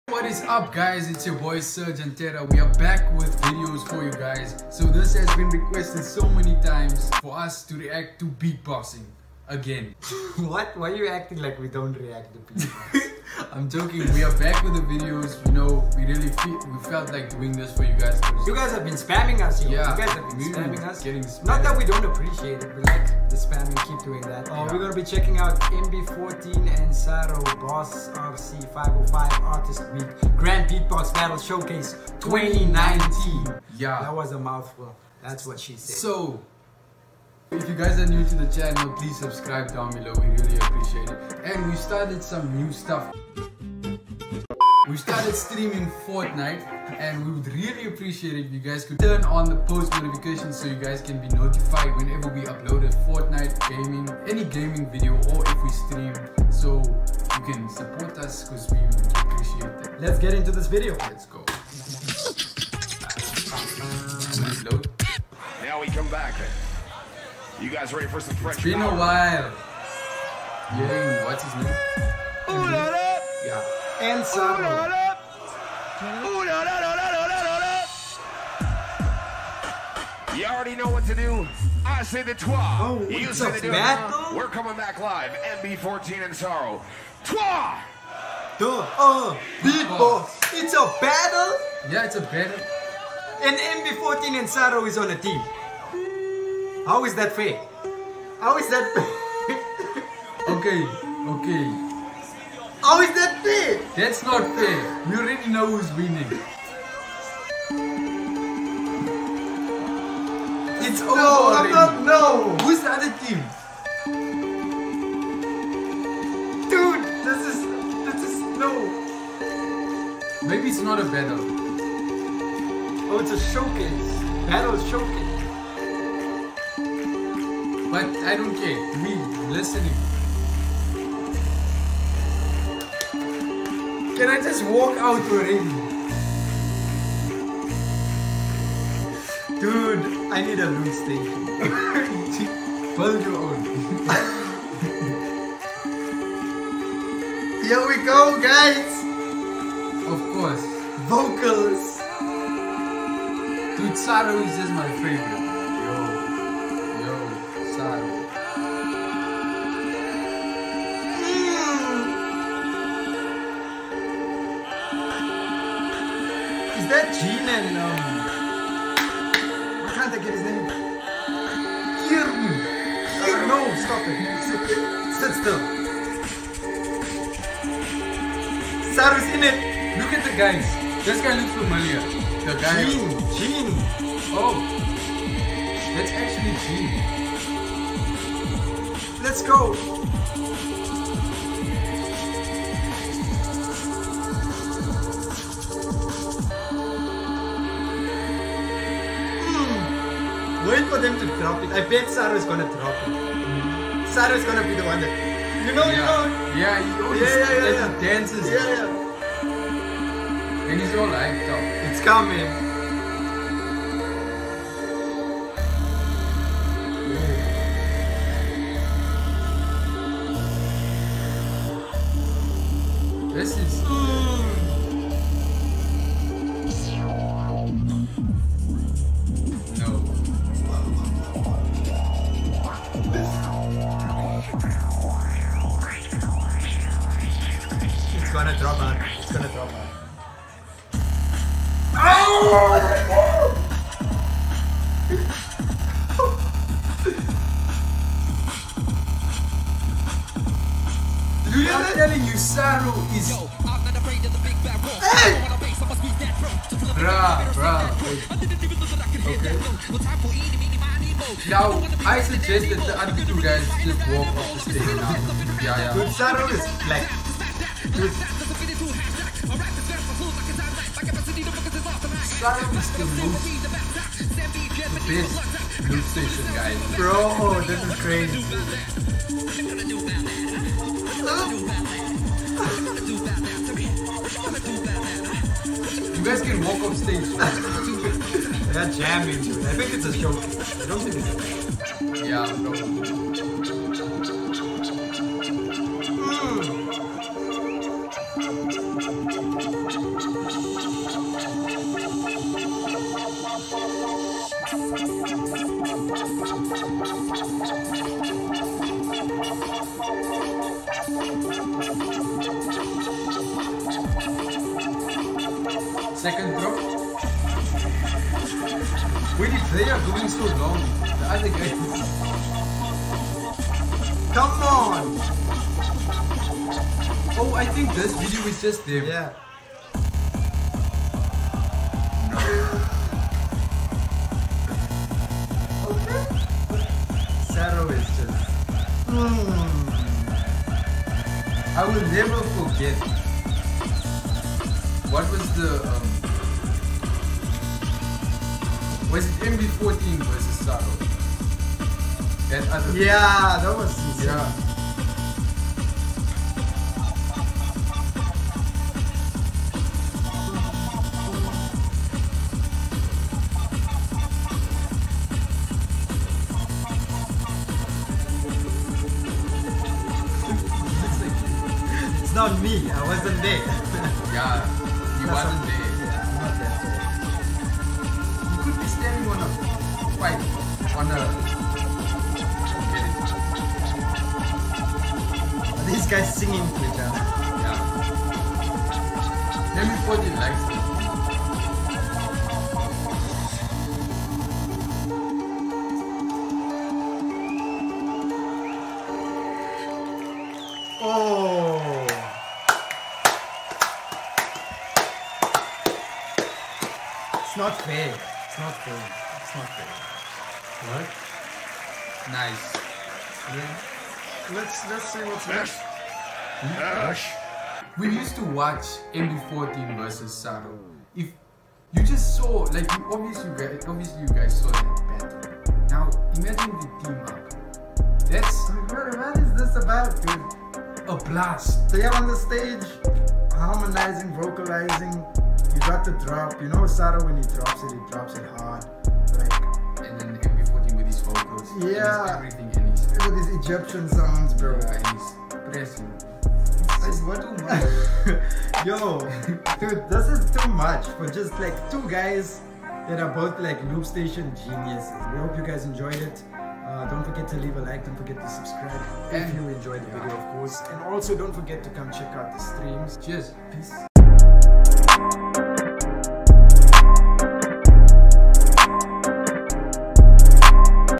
MB14 & SARO | Boss Rc-505 Artist Week | GBB Showcase 2019 || REACTION
This is a dope Loopstation Duo, it's also very OP if you think about it!

Camera We Use:
Headsets We Use:
▶️ Steelseries Arctis 3's
7.1 Surround Gaming Headsets

Video Uploaded By:
SerGe/Terra/SerGe&Terra/S&T